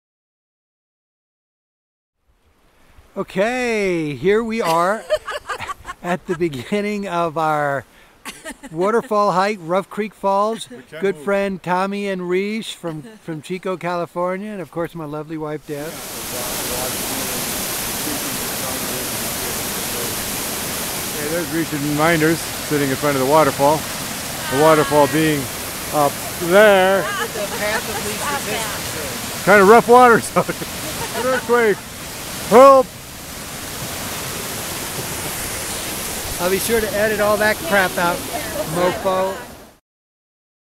Picking Up Where We Left Off
Visiting with friends, Tommy and Risha from Chico, CA in April, 2010